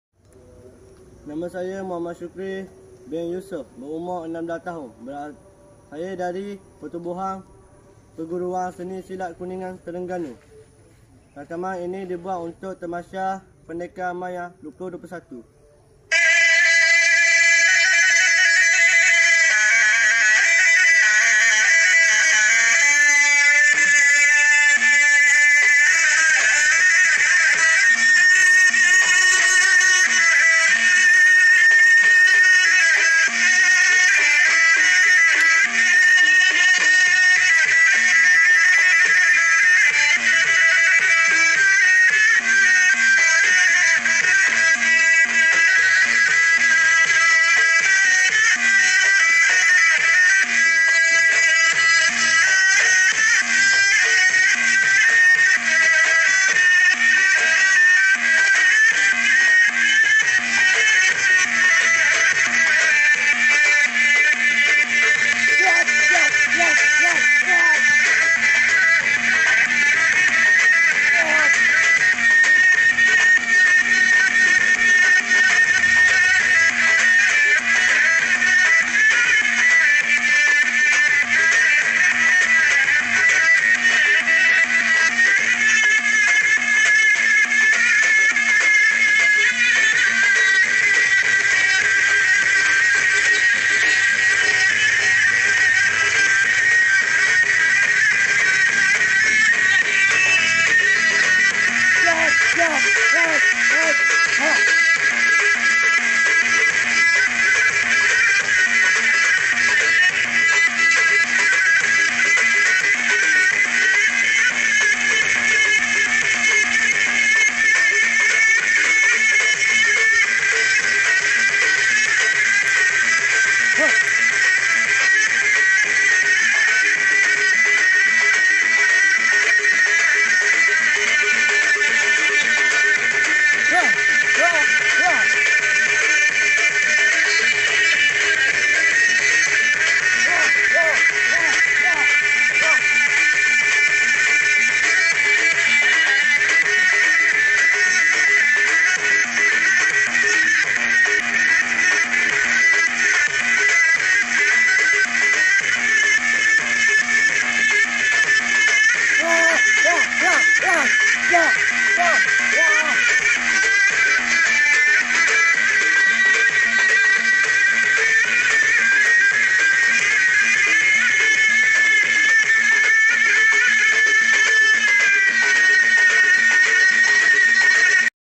Seni Silat Solo Kreatif - Muhammad Syukri Bin Yusoff
Video peserta Pertandingan Seni Silat Solo Kreatif Sempena Temasya Pendekar Maya 2021

Nama : Muhammad Syukri Bin Yusoff

Kategori : Remaja

Persatuan : Pertubuhan Perguruan Seni Silat Kuningan Terengganu

Negeri : Terengganu